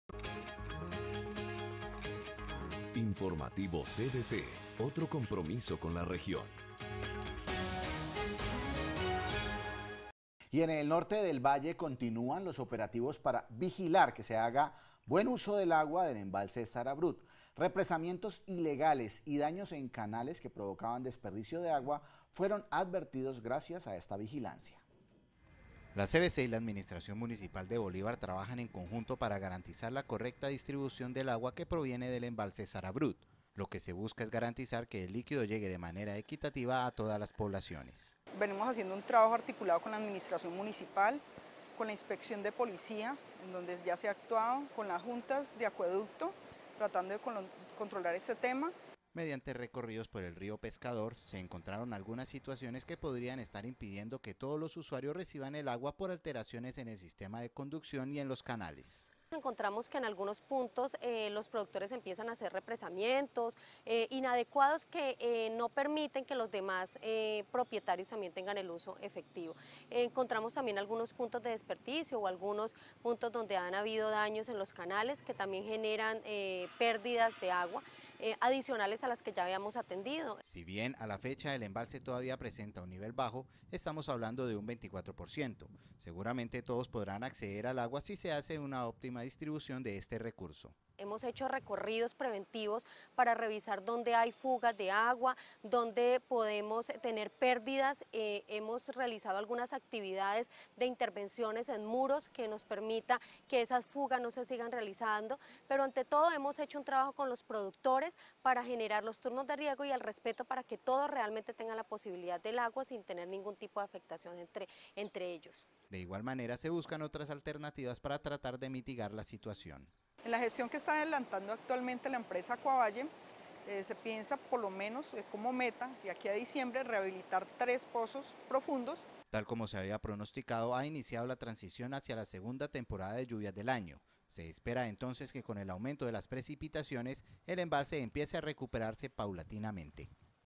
SIGUEN OPERATIVOS EN EL BRUT
Y en el norte del Valle continúan los operativos para vigilar que se haga buen uso del agua del embalse Sara Brut.
Represamientos ilegales y daños en canales que provocaban desperdicio de agua fueron advertidos gracias a esta vigilancia.